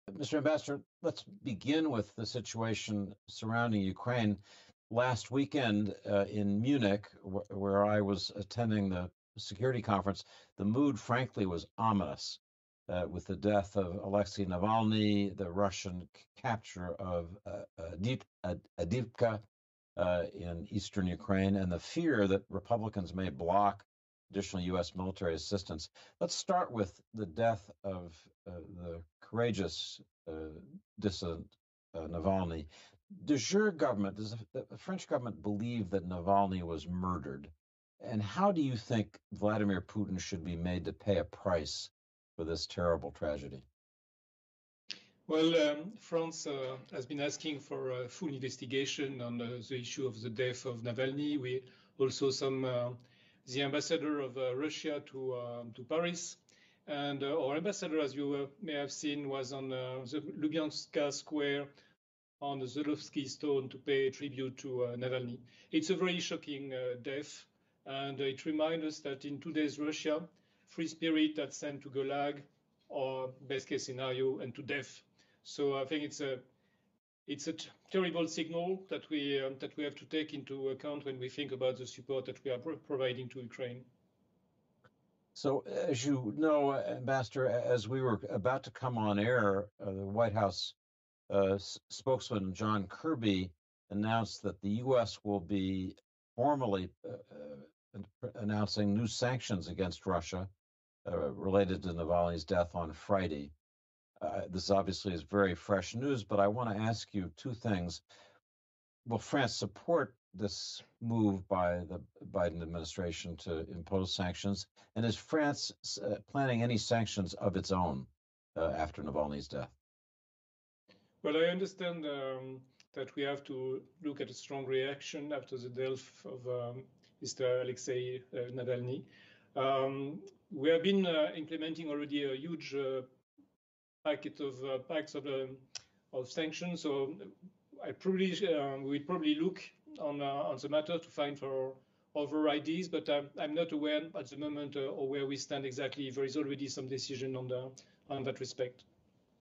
French Ambassador on death of Alexei Navalny, new sanctions
“France has been asking for a full investigation on the issue of the death of Navalny…. It’s a very shocking death…. I understand that we have to look at a strong reaction after the death of Mr. Alexei Navalny. We have been implementing already a huge package of sanctions, so we’ll probably look on the matter … but I’m not aware at the moment where we stand exactly.” –Laurent Bili